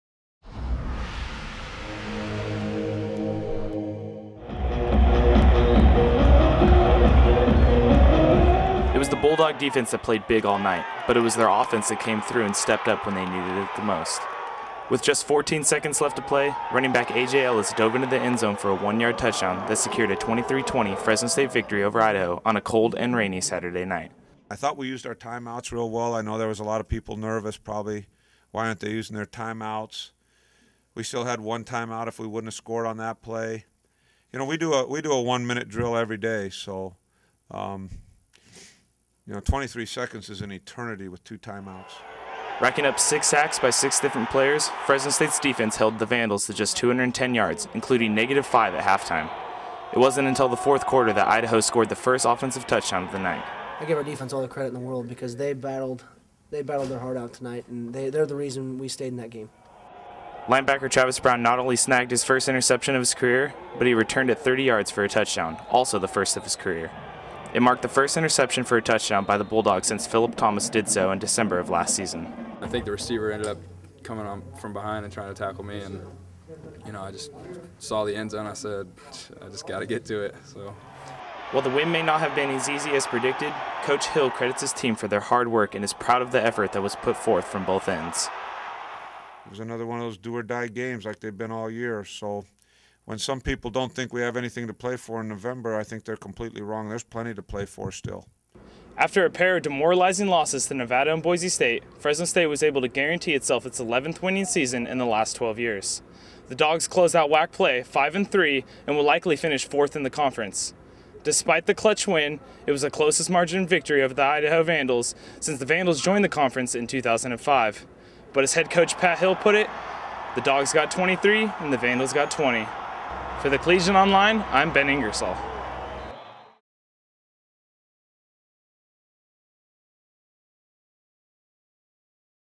Fresno State vs. Idaho Recap 11/28/2010
The Bulldogs dodge a possible upset to WAC opponent Idaho in a close but crucial win.